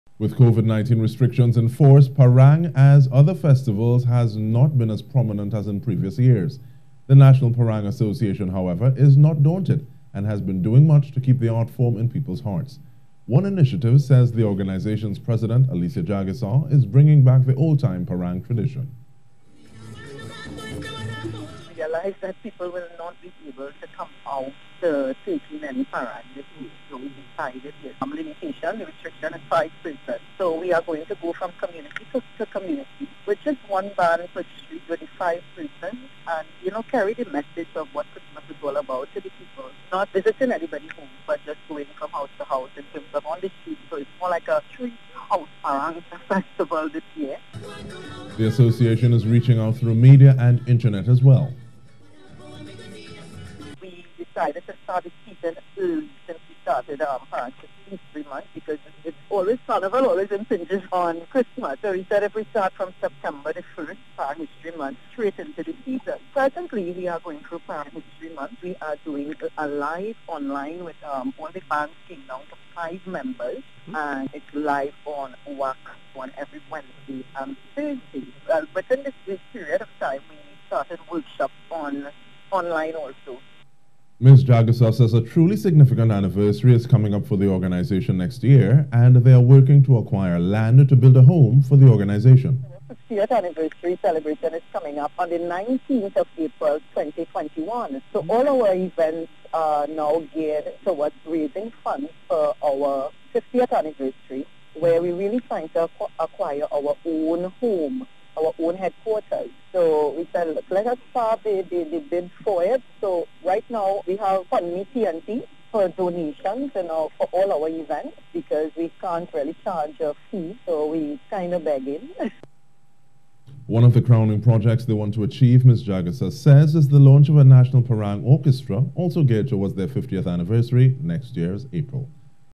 A Different Parang Season
With COVID-19 restrictions in force, parang, as other festivals, has not been as prominent as in previous years.

The National Parang Association however is not daunted and has been doing much to keep the art form in people's hearts.

Association President, Alicia Jaggassar is bringing back the ol' time parang tradition.

The Association is reaching out through media and internet as well.

Ms. Jaggassar says a truly significant anniversary is coming up for the organisation next year, and they are working to acquire land to build a home for the organisation.

One of the crowning projects they want to achieve is the launch of a National Parang Orchestra, also geared toward their 50th anniversary next year.